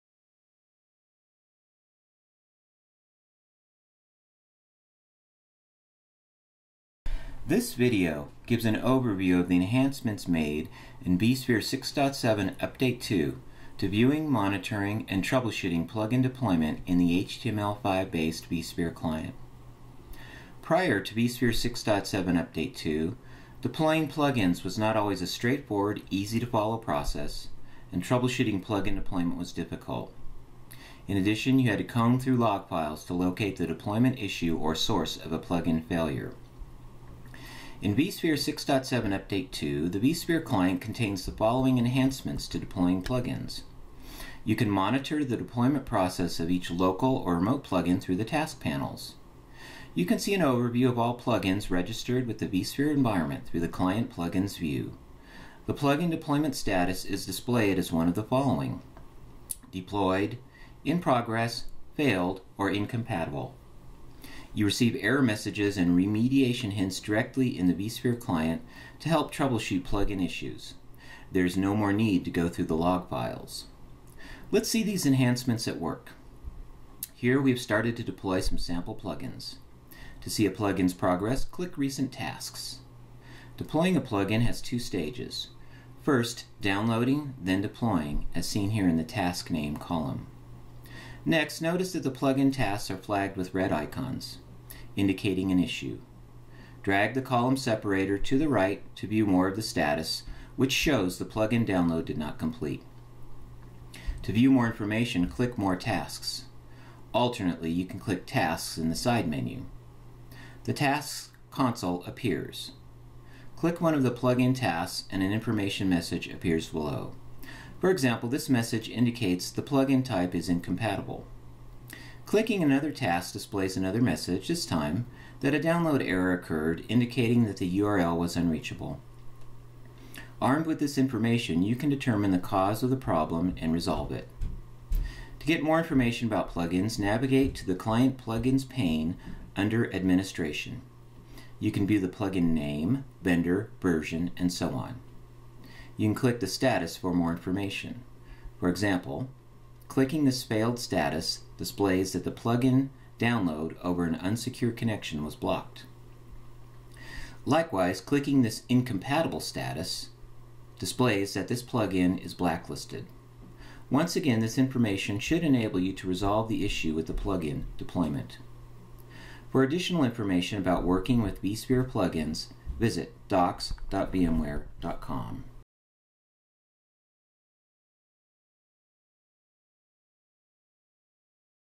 Enhancements to Working with Plug-ins in the vSphere Client (vSphere 6.7 Update 2)
This video gives a brief overview of the enhancements made in vSphere 6.7 Update 2 to viewing, monitoring, and troubleshooting plug-in deployment in the HTML5-based vSphere Client.